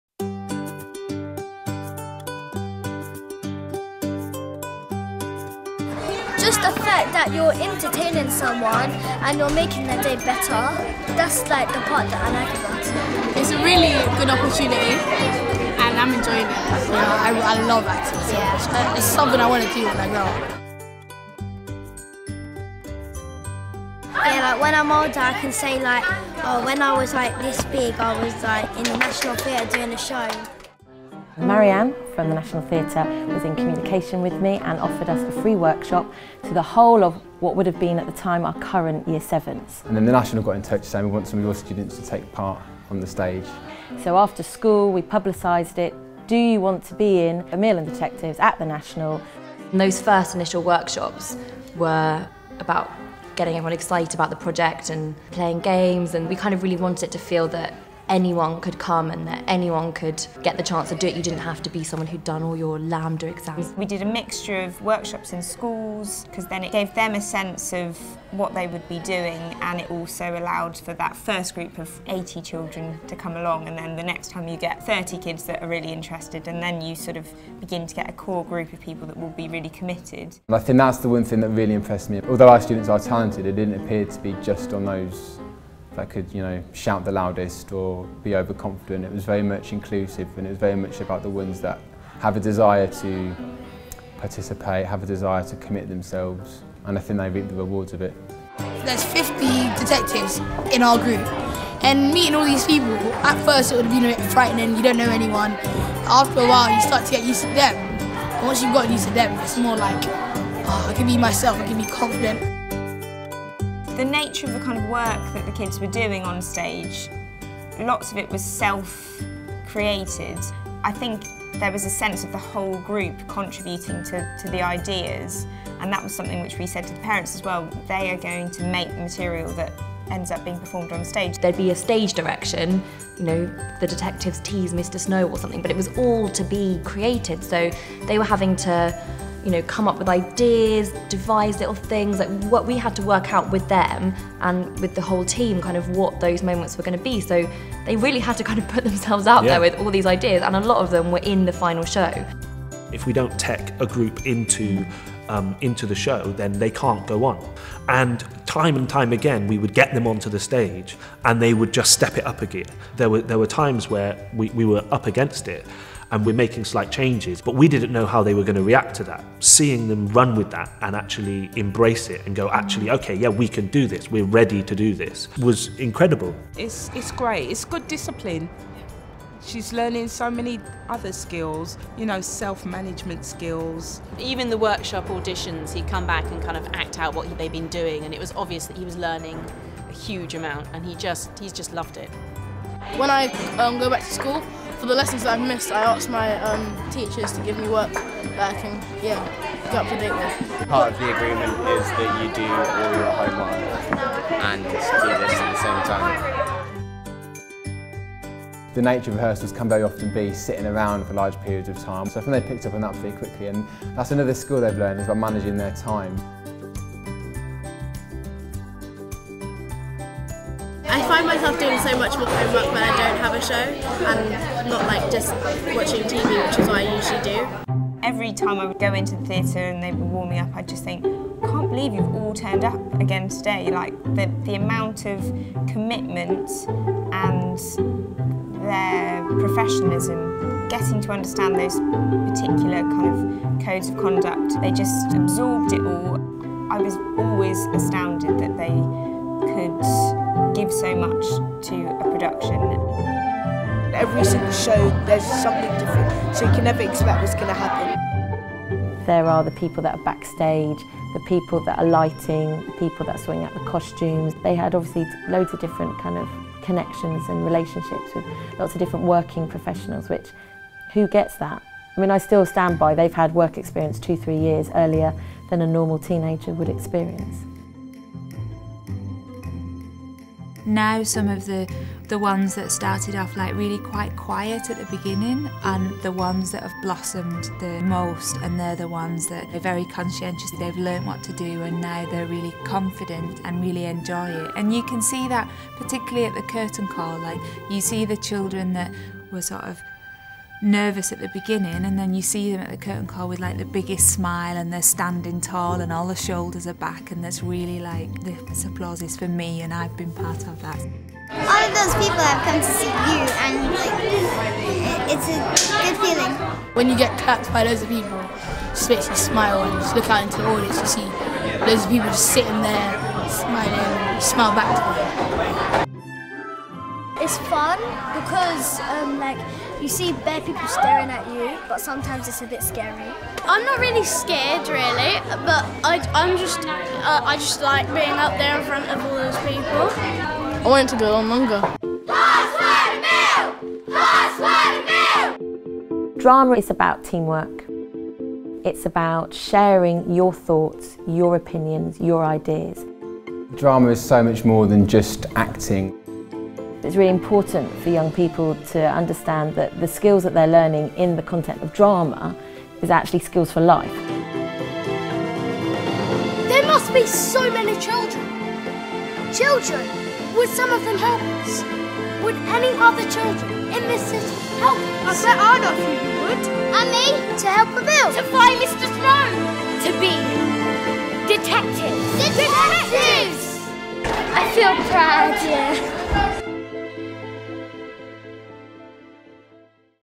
Every Child Should Have The Opportunity To Do Drama
A short film about the local children who played the detectives in the National Theatre's 2013 production of Emil and the Detectives.

At a time when Drama in the English national curriculum is being reduced and fewer teachers are qualifying to teach drama, this film seeks to show just how important drama is and the positive impact it can have on young people.